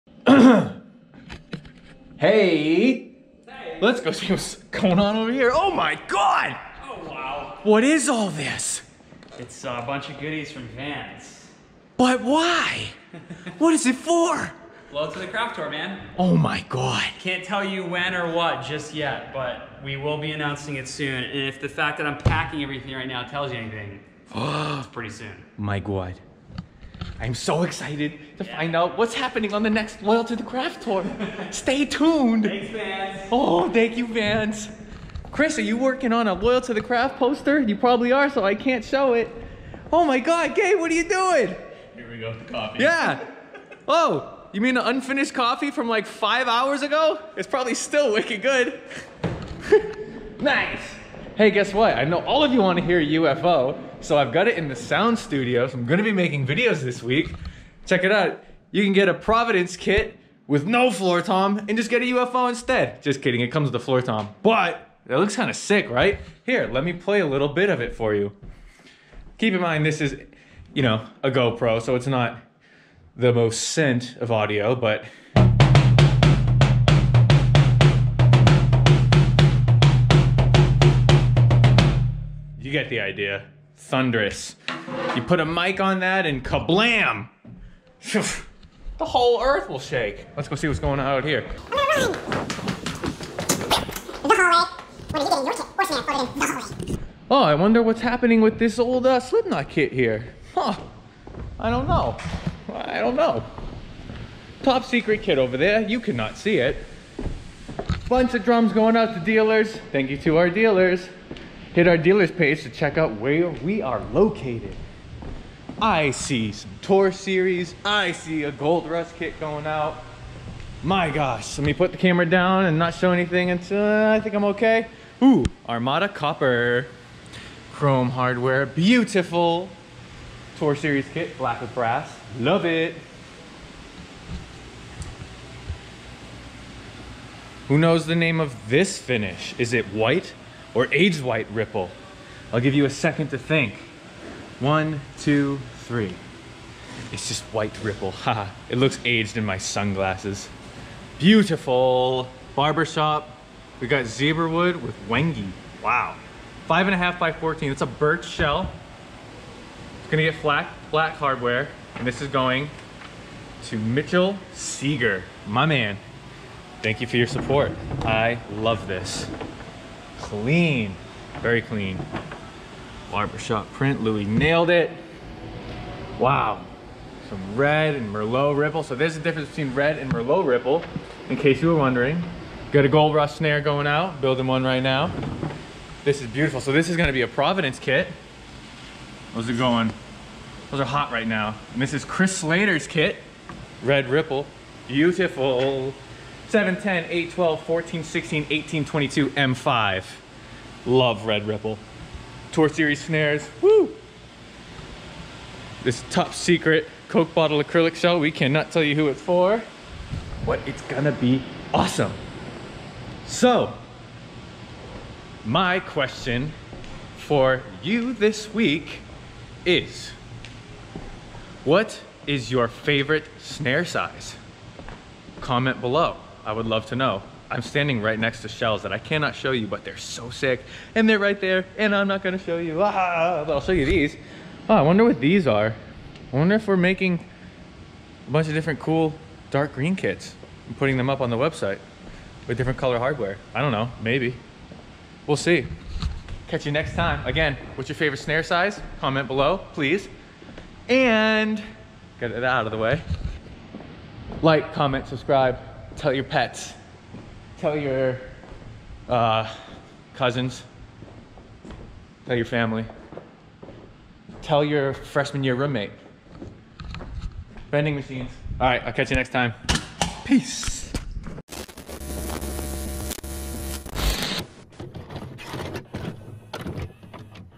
SJC Custom Drums - Vlog Update! New sunglasses and favorite snare size?!?!
WHAT IS YOUR FAVORITE SNARE SIZE! Let us know below!!!

Gabe never finishes his coffees... what the heck...

AND wow, so many awesome builds coming through the shop! Want to go custom? YES, you do... so click below!!!